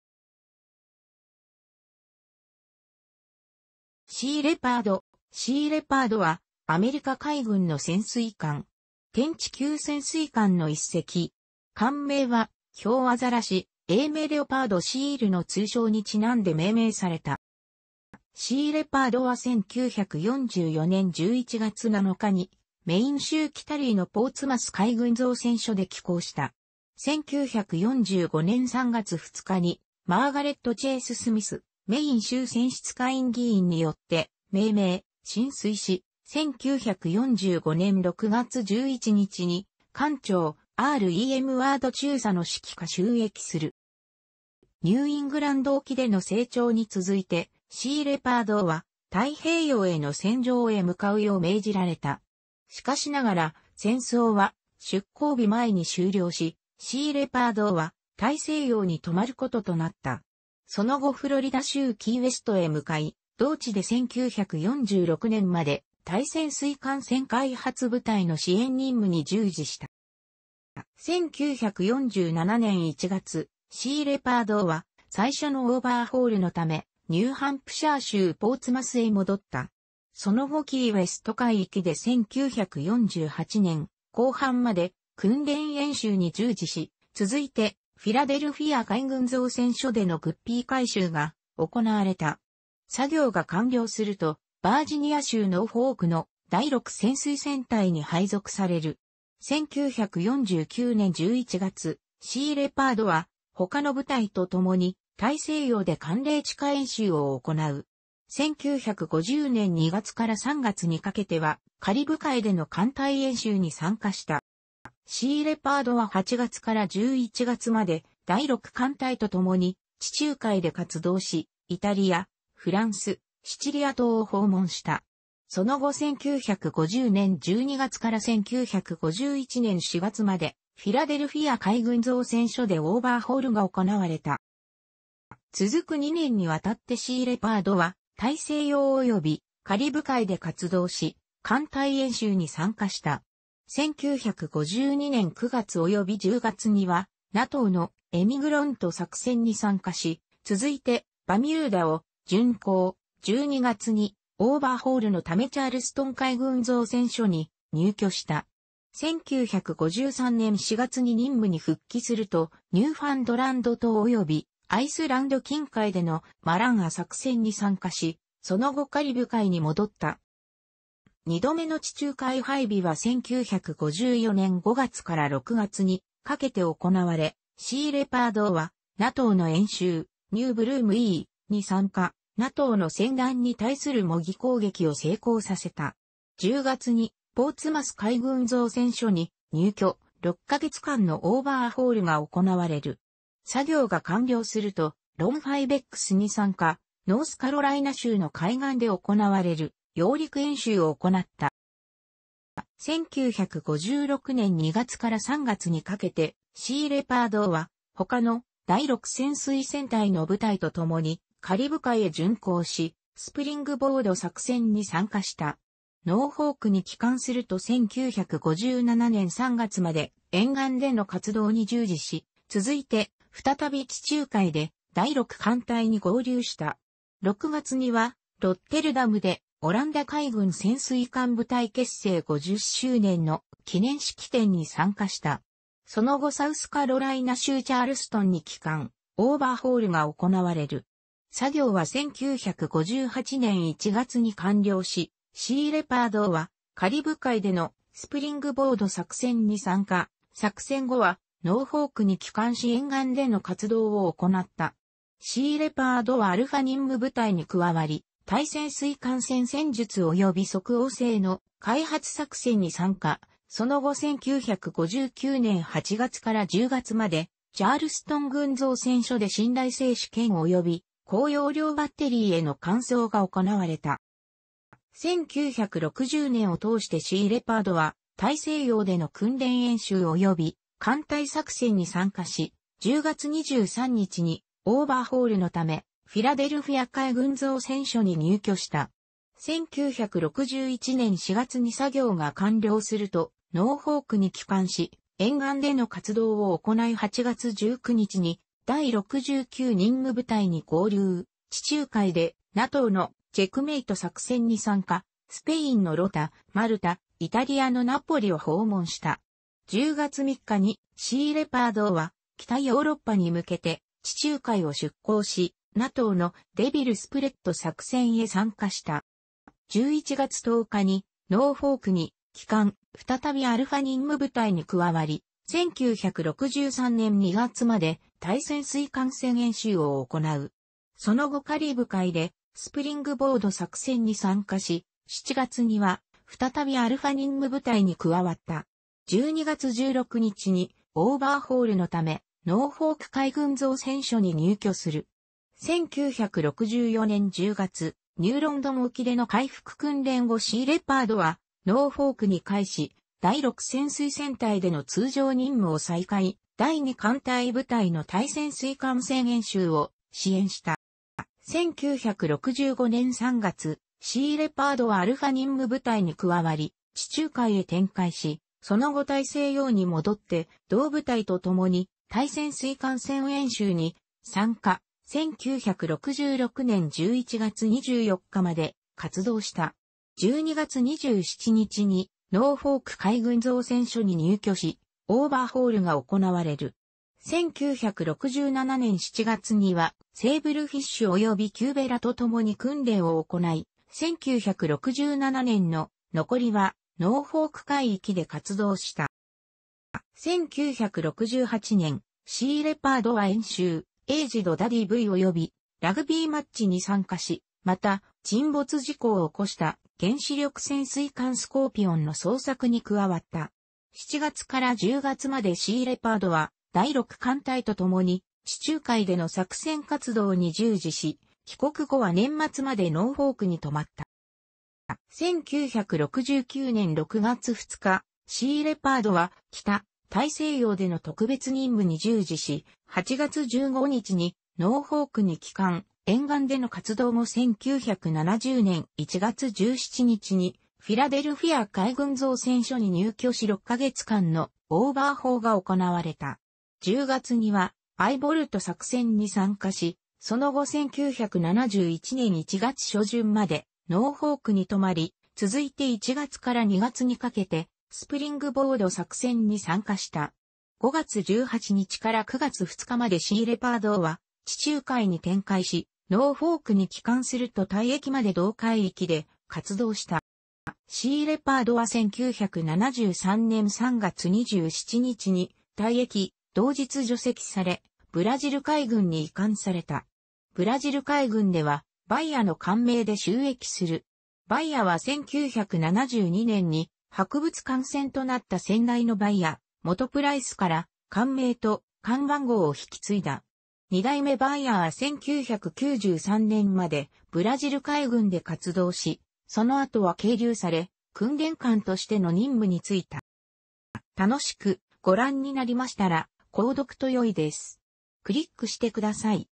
シーレパード (潜水艦)
シーレパード (USS Sea Leopard, SS-483) は、アメリカ海軍の潜水艦。テンチ級潜水艦の一隻。艦名はヒョウアザラシ（英名Leopard seal）の通称に因んで命名された。

シーレパードは1944年11月7日にメイン州キタリーのポーツマス海軍造船所で起工した。1945年3月2日にマーガレット・チェース・スミス（メイン州選出下院議員）によって命名、進水し、1945年6月11日に艦長R・E・M・ワード中佐の指揮下就役する。

ニューイングランド沖での整調に続いて、シーレパードは太平洋への戦場へ向かうよう命じられた。しかしながら戦争は出航日前に終了し、シーレパードは大西洋に留まることとなった。その後フロリダ州キーウェストへ向かい、同地で1946年まで対潜水艦戦開発部隊の支援任務に従事した。

1947年1月、シーレパードは最初のオーバーホールのためニューハンプシャー州ポーツマスへ戻った。その後キーウェスト海域で1948年後半まで訓練演習に従事し、続いてフィラデルフィア海軍造船所でのGUPPY 改修が行われた。作業が完了するとバージニア州ノーフォークの第6潜水戦隊に配属される。1949年11月、シーレパードは他の部隊と共に大西洋で寒冷地下演習を行う。1950年2月から3月にかけてはカリブ海での艦隊演習に参加した。

シーレパードは8月から11月まで第6艦隊と共に地中海で活動し、イタリア、フランス、シチリア島を訪問した。その後1950年12月から1951年4月までフィラデルフィア海軍造船所でオーバーホールが行われた。

続く2年にわたってシーレパードは大西洋およびカリブ海で活動し、艦隊演習に参加した。1952年9月および10月にはNATOの「"Emigrant"」作戦に参加し、続いてバミューダを巡航、12月にオーバーホールのためチャールストン海軍造船所に入渠した。1953年4月に任務に復帰すると、ニューファンドランド島およびアイスランド近海での「"Mariner"」作戦に参加し、その後カリブ海に戻った。

2度目の地中海配備は1954年5月から6月にかけて行われ、シーレパードはNATOの演習「"New Broom II"」に参加、NATOの船団に対する模擬攻撃を成功させた。10月にポーツマス海軍造船所に入渠、6ヶ月間のオーバーホールが行われる。作業が完了すると「"LantPhibEx"」に参加、ノースカロライナ州の海岸で行われる揚陸演習を行った。

1956年2月から3月にかけてシーレパードは他の第6潜水戦隊の部隊と共にカリブ海へ巡航し、「"Springboard"」作戦に参加した。ノーフォークに帰還すると1957年3月まで沿岸での活動に従事し、続いて再び地中海で第6艦隊に合流した。6月にはロッテルダムでオランダ海軍潜水艦部隊結成50周年の記念式典に参加した。その後サウスカロライナ州チャールストンに帰還、オーバーホールが行われる。作業は1958年1月に完了し、シーレパードはカリブ海での「"Springboard"」作戦に参加、作戦後はノーフォークに帰還し沿岸での活動を行った。シーレパードはアルファ任務部隊に加わり対潜水艦戦戦術および即応性の開発作戦に参加、その後1959年8月から10月までチャールストン海軍造船所で信頼性試験および高容量バッテリーへの換装が行われた。

1960年を通してシーレパードは大西洋での訓練演習および艦隊作戦に参加し、10月23日にオーバーホールのためフィラデルフィア海軍造船所に入渠した。1961年4月に作業が完了するとノーフォークに帰還し、沿岸での活動を行い8月19日に第69任務部隊に合流、地中海でNATOの「"Checkmate"」作戦に参加、スペインのロタ、マルタ、イタリアのナポリを訪問した。10月3日にシーレパードは北ヨーロッパに向けて地中海を出航し、NATOの「"Devil Spread"」作戦へ参加した。

11月10日にノーフォークに帰還、再びアルファ任務部隊に加わり、1963年2月まで対潜水艦戦演習を行う。その後カリブ海で「"Springboard"」作戦に参加し、7月には再びアルファ任務部隊に加わった。12月16日にオーバーホールのためノーフォーク海軍造船所に入渠する。1964年10月、ニューロンドン沖での回復訓練後シーレパードはノーフォークに帰還し、第6潜水戦隊での通常任務を再開、第2艦隊部隊の対潜水艦戦演習を支援した。

1965年3...